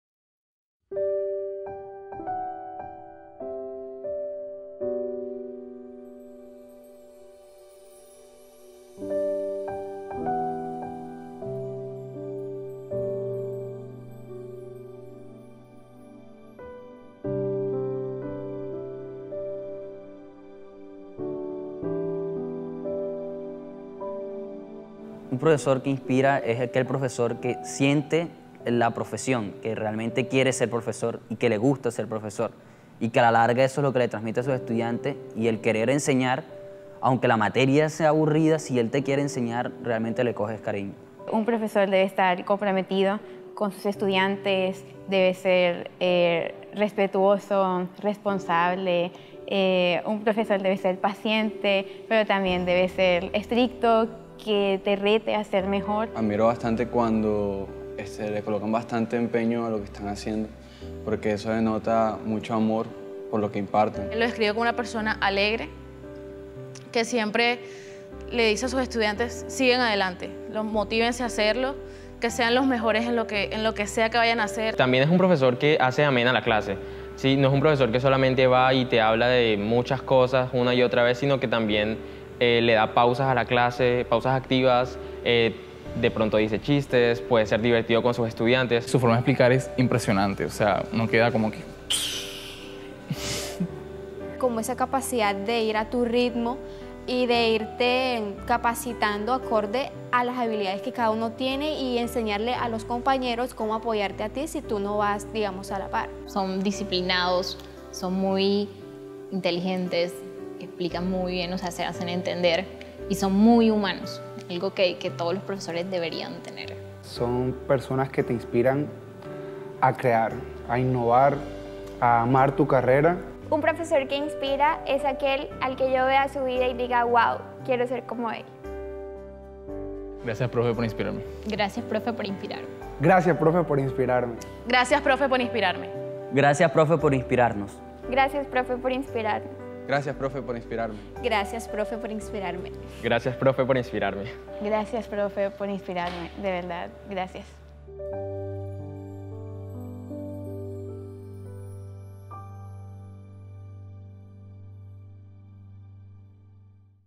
MiProfeMeInspira - ¡Feliz día profes!
Le preguntamos a un grupo de estudiantes de la Universidad del Norte sobre las características que convierten un profesor convencional a un profesor que inspira y estas fueron sus respuestas: